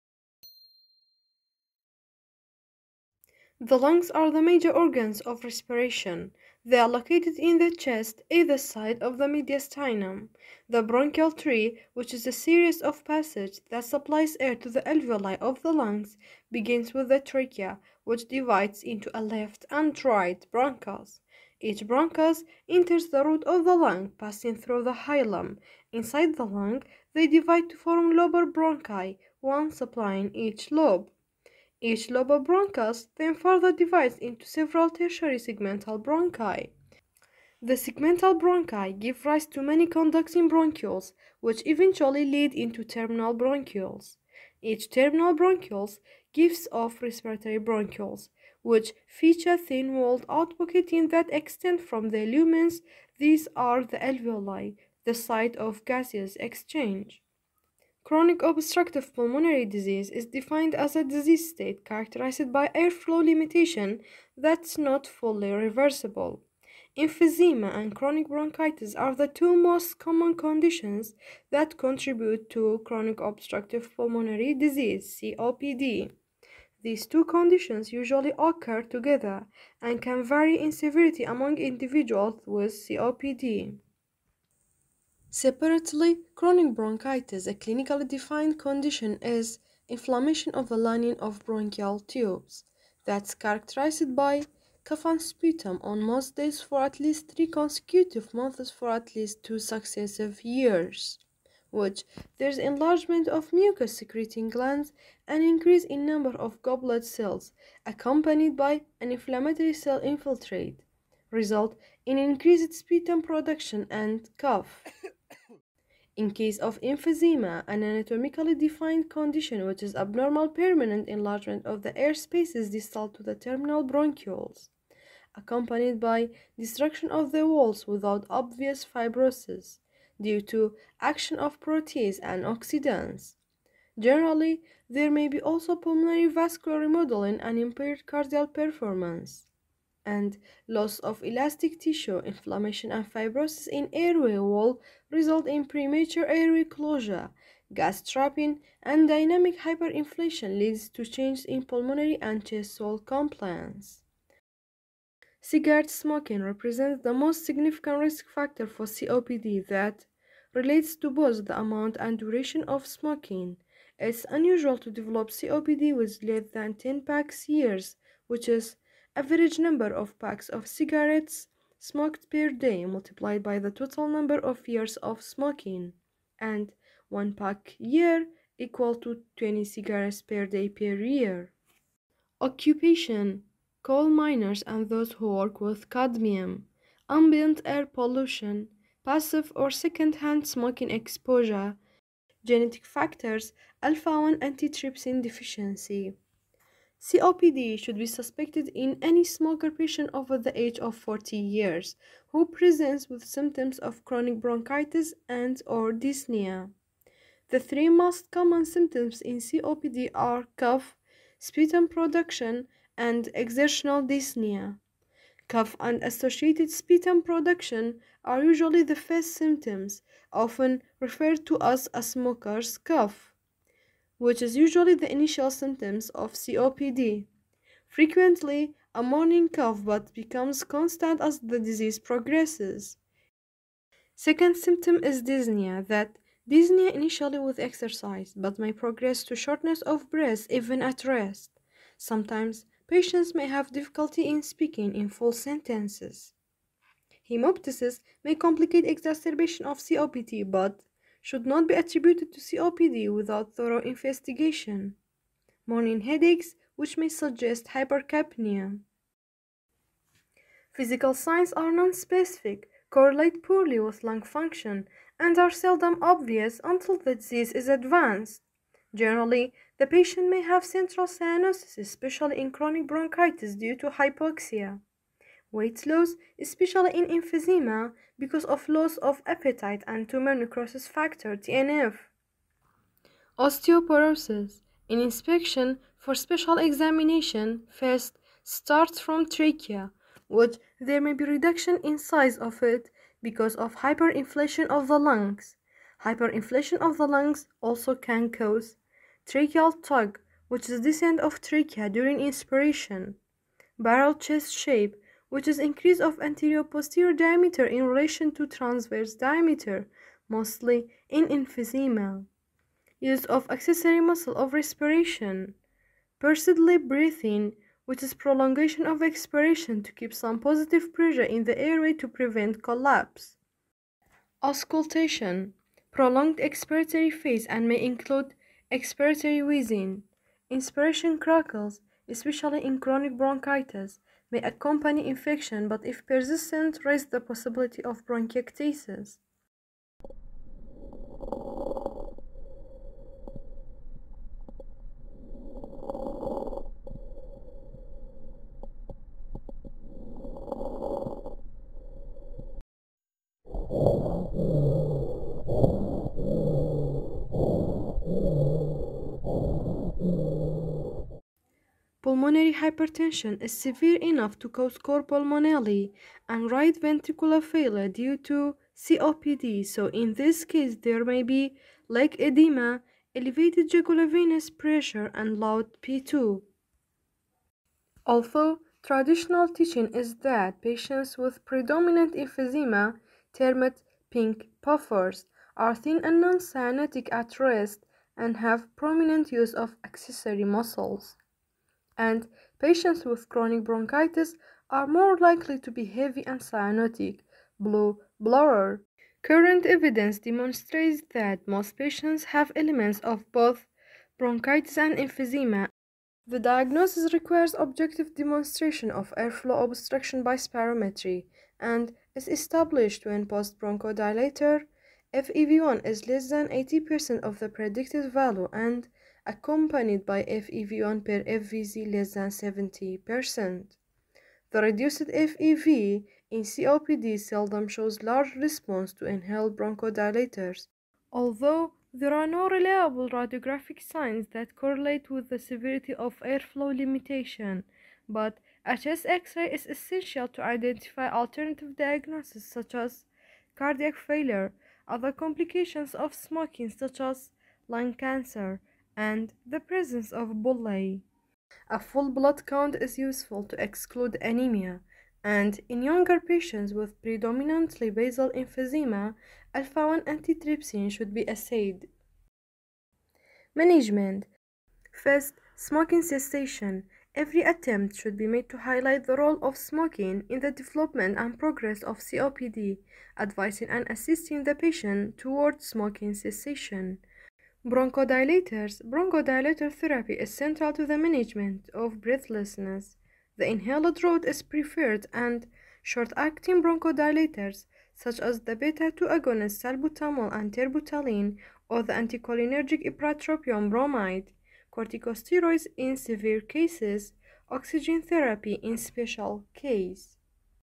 COPD (Anatomy -Pathophysiology-History-Physical Sign -Investigation-Management)
✔️Content ...

1-Anatomy of the lungs 00:04
2-COPD definition 00:59
3- Chronic bronchitis Pathophysiology 01:26
4- Emphysema Pathophysiology 01:56
5- Risk Factors 02:35
6-  History 03:18
7- Physical Sign 04:20
8-Investigation 06:47
9-Management 07:43